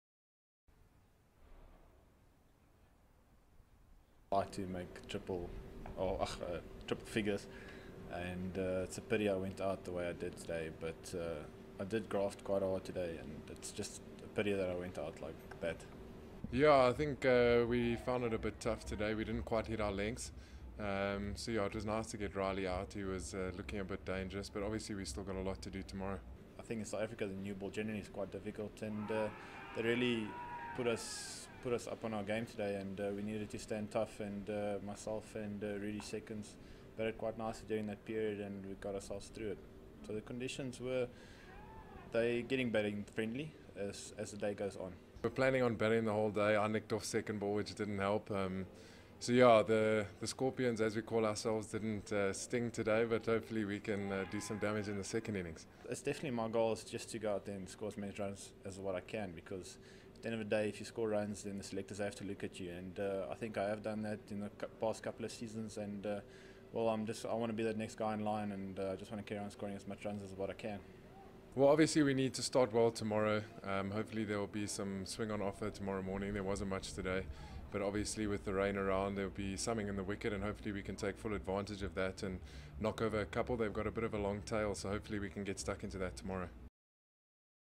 VAN DER WATH BLITZ PULLS WARRIORS BACK
JOHAN van der Wath's four-wicket blitz in the morning and Rilee Rossouw's half-century in the afternoon put the Chevrolet Knights in control of their Sunfoil Series match against the Chevrolet Warriors on day two at Chevrolet Park. Van der Wath finished with figures of 6 for 74 as the Warriors were bowled out for 274 in their first innings, before Rossouw hit a brisk 52 to leave the Knights on 165 for three when rain brought a premature end to the day.